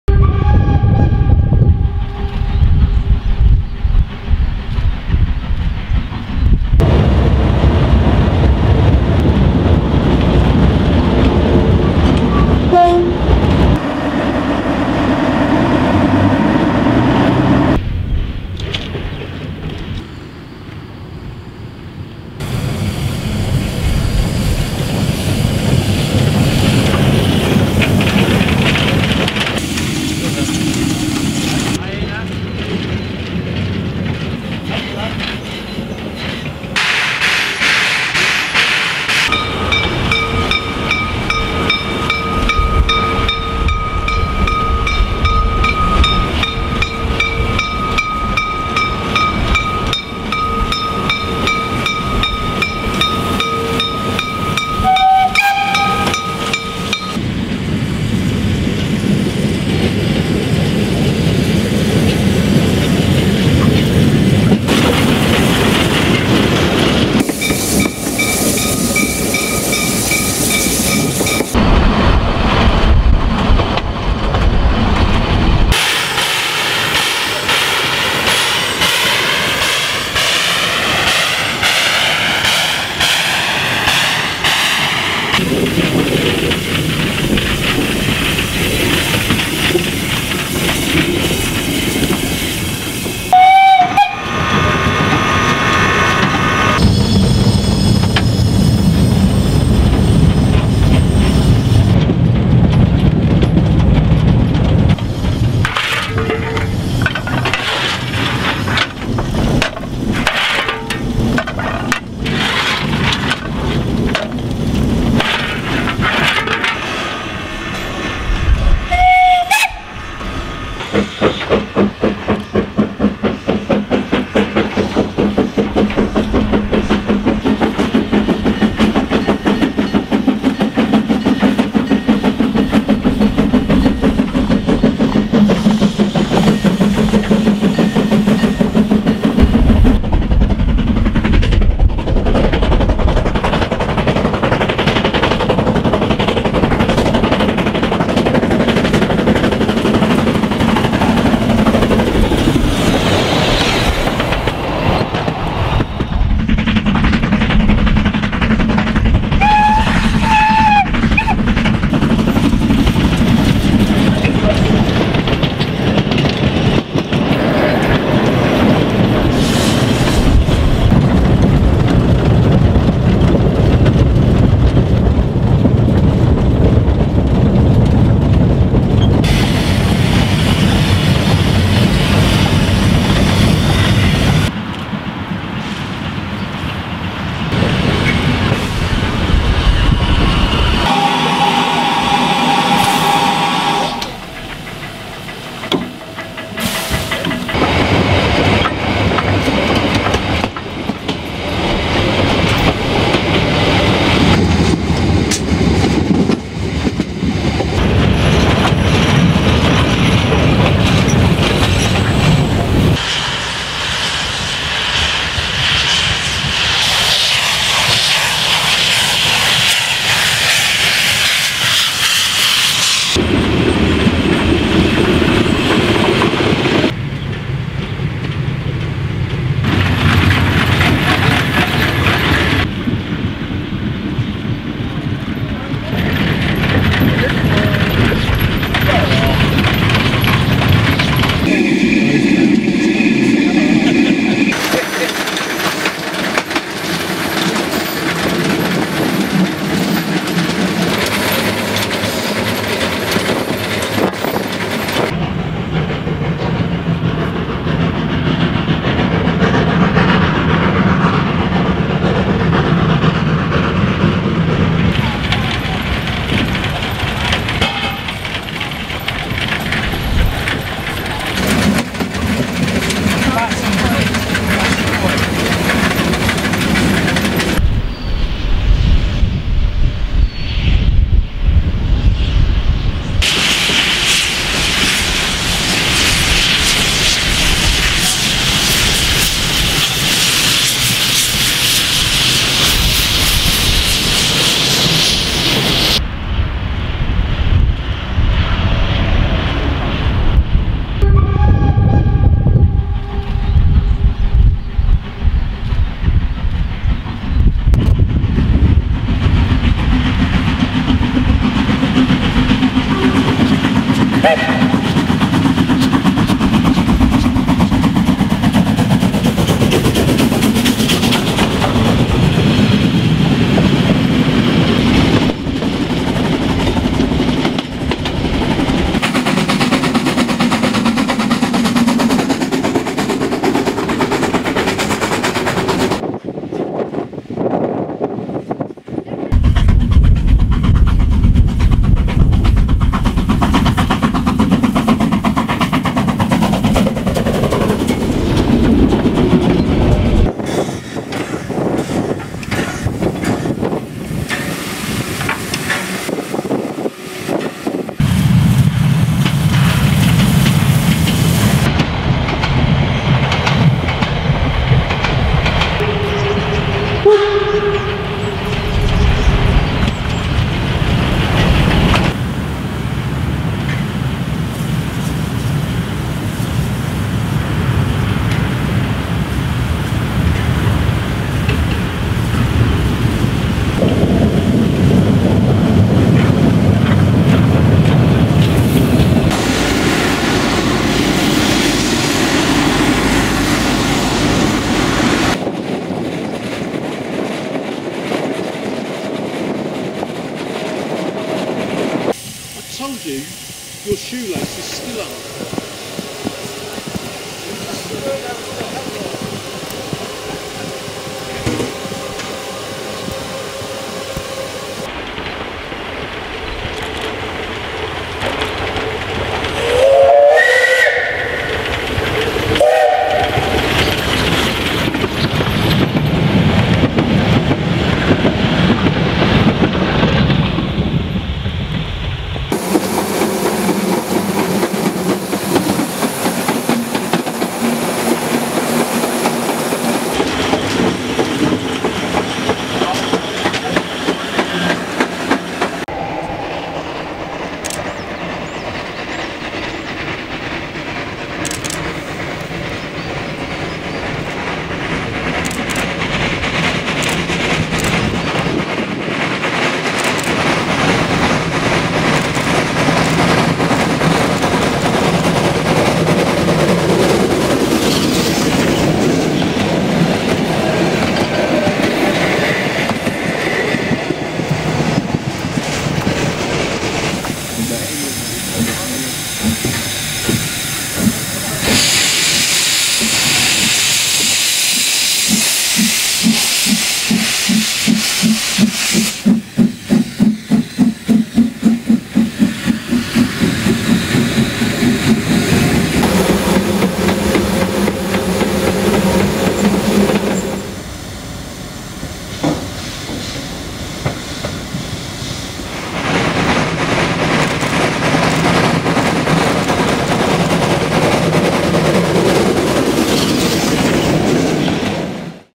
Highlights of Germany
Short version of Railway Touring Company  holiday, February 2017.  the trip included some good tram rides but these didn't make the video cut.